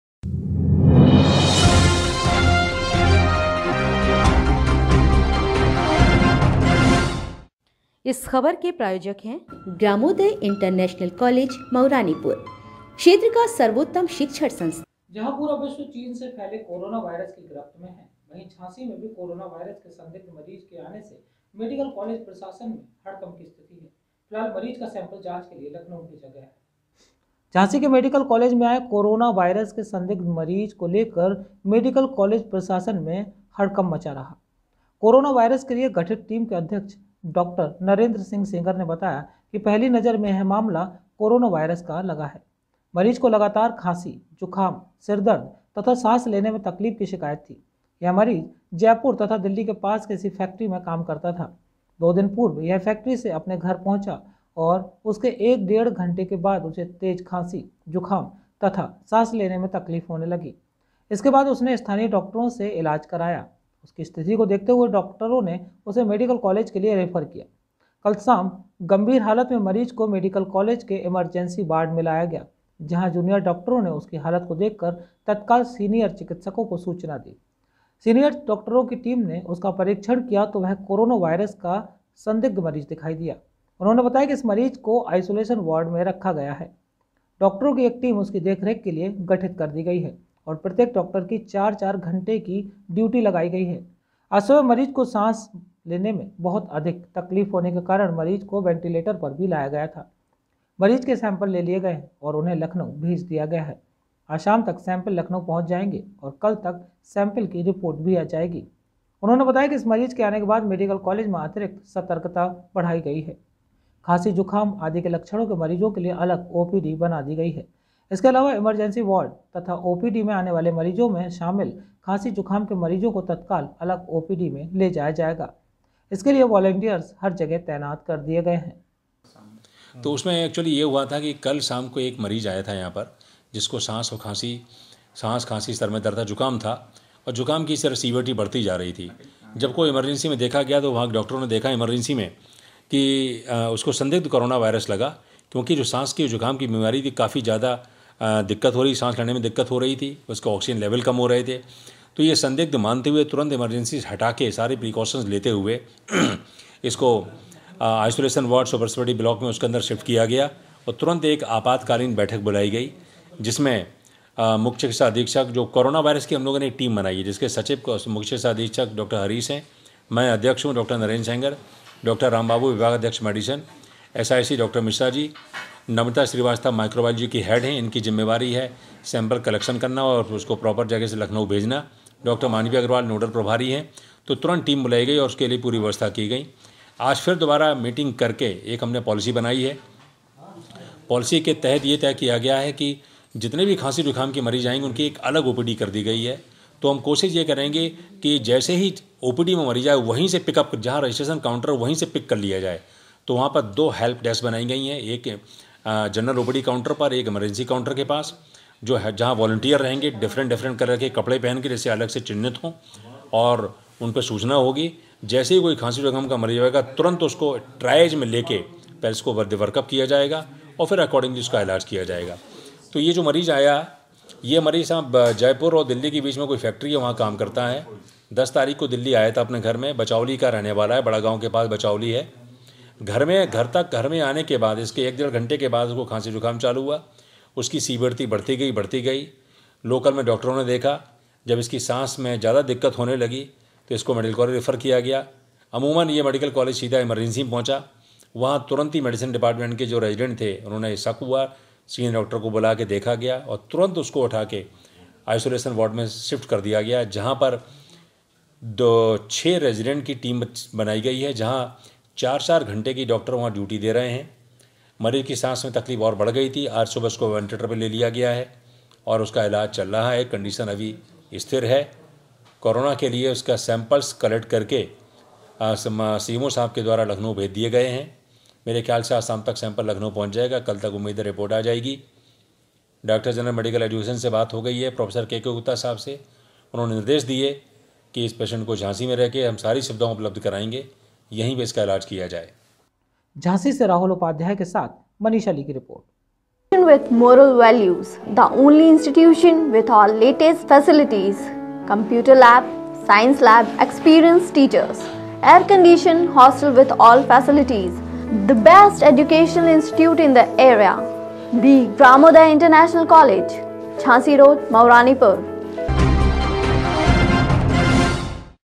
सावधान: झाँसी में कोरोना वायरस का संदिग्ध मरीज, मेडीकल काॅलेज में भर्ती।
Report : Manish Ali & Rahul Upadhyay
Jhansi